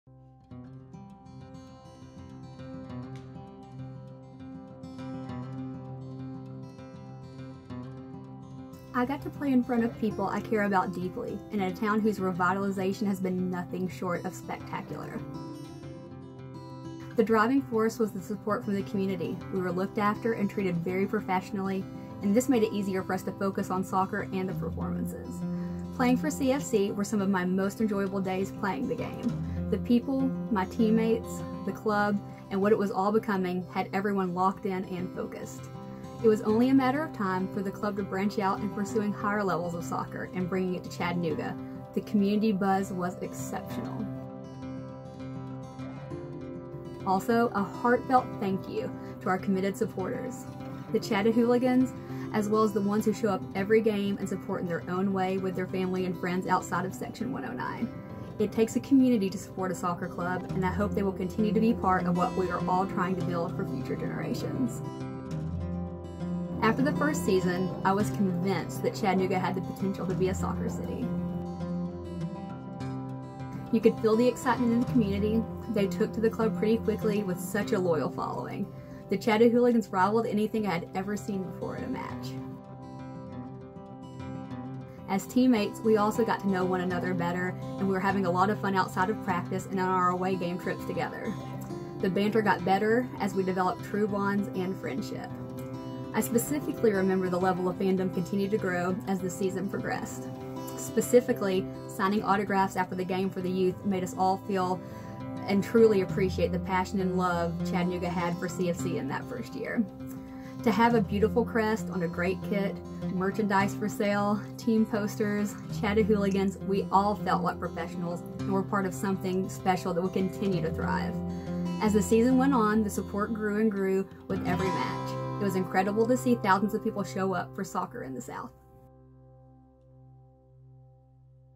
Founder's Day - Looking Back
Players from our inaugural team look back at their memories of the first season in Club history